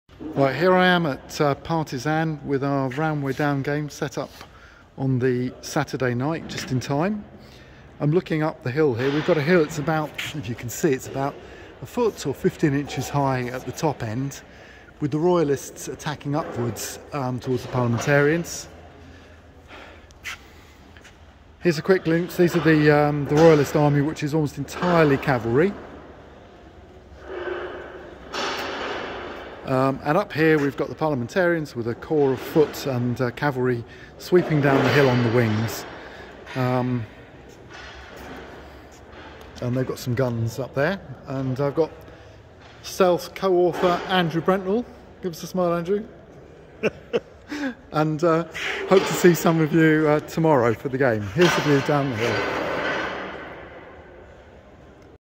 Battle of Roundway Down
Here's a brief preview of the Battle of Roundway Down which Andrew Brentnall and I will be running at Partizan in Newark-on-Trent tomorrow. The hill it is on us 12' long and over 1' high at the top- but there's a good chance that Royakust elan will get them to the top!